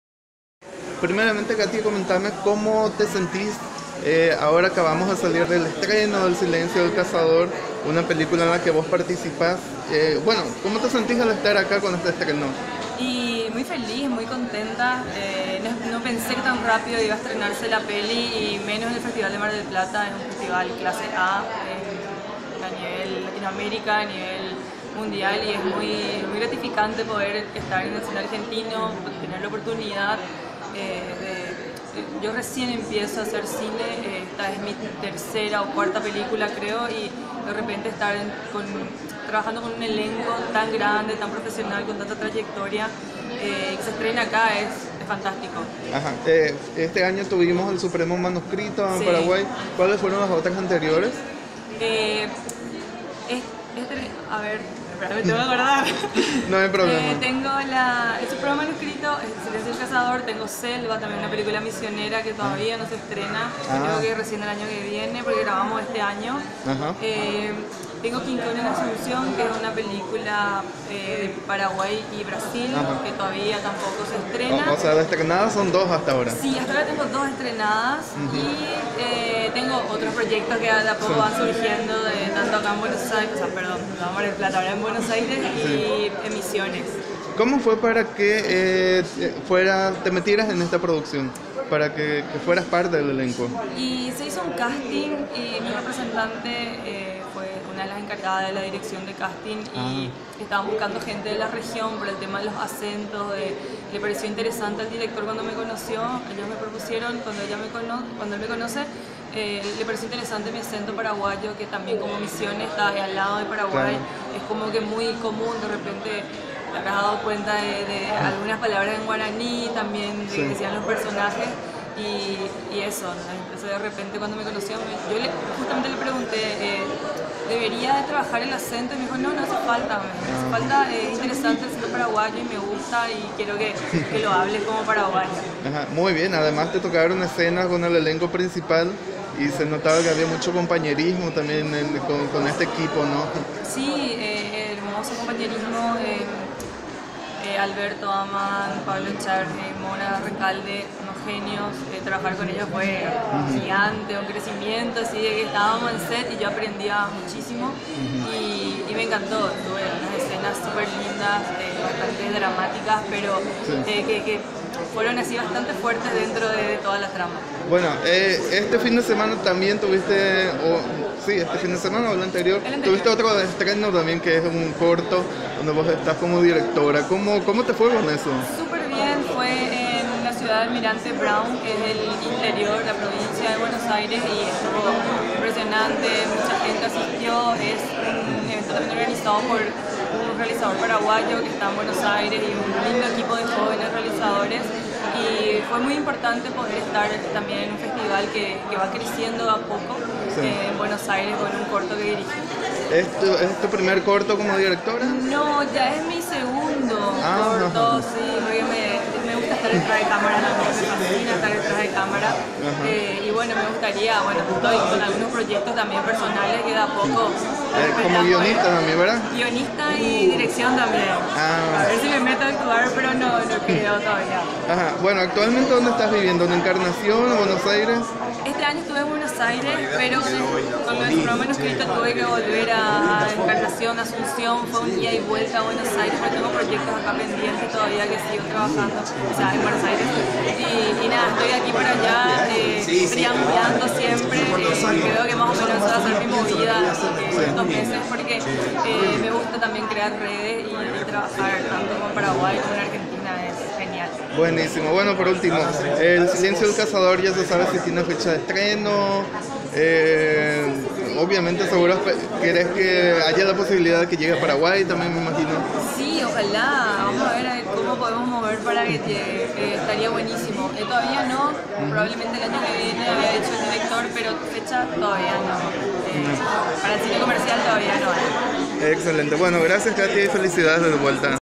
34MDQFilmFest Katia Acosta sobre "El silencio del cazador"
34º Festival Internacional de Cine de Mar del Plata. Argentina. 9 al 18 de noviembre de 2019. Sitio: mardelplatafilmfest.com.